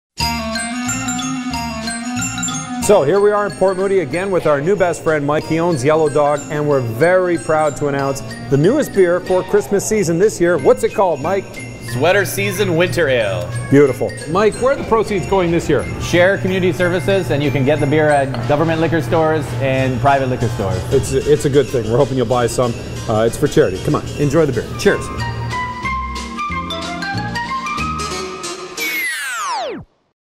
JOS x Yellow Dog - Sweater Season Winter Ale
The Jeff O'Neil Show at CFOX teamed up with Yellow Dog Brewing to bring you the best beer of Sweater Season!
This year, Yellow Dog has crafted a winterized version of their porter. They added vanilla, cinnamon, nutmeg, and top it all off, they threw in some lactose to sweeten it up a bit.
The result will hug you like that warm holiday sweater you look forward to every winter.